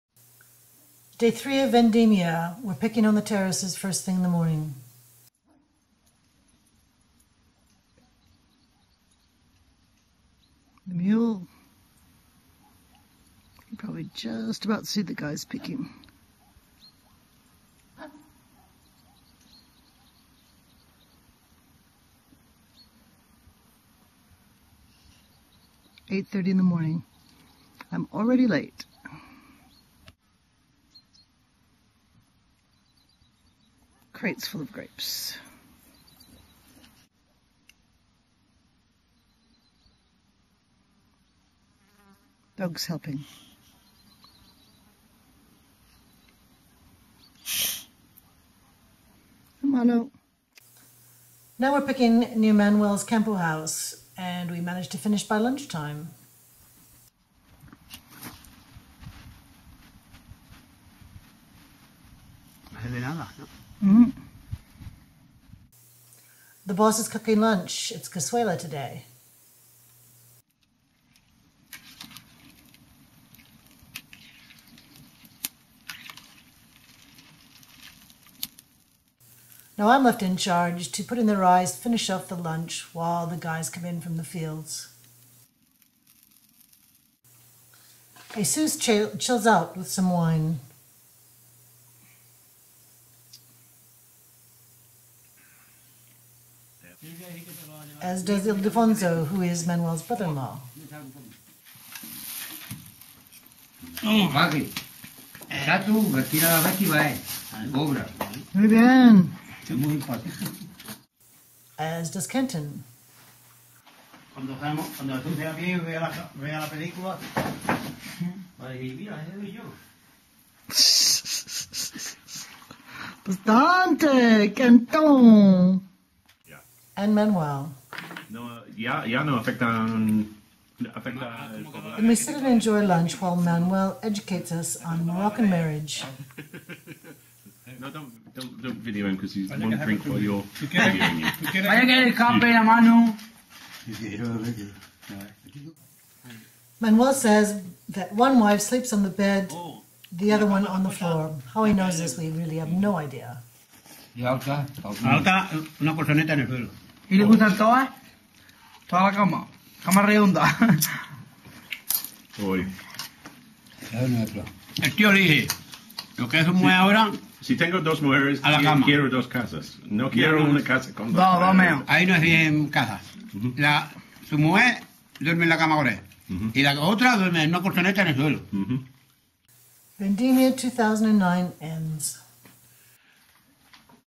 Vendimia 2009 - grape picking
The grape picking was short but hard this year - hot and sunny. The grapes are full of juice, so it should be a good wine! In this video, you meet the workers - Jesus, his father Antonio, Ildefonso and Terry, our neighbour. Also you have the usual suspects - Kenton, Manuel, the dogs and me!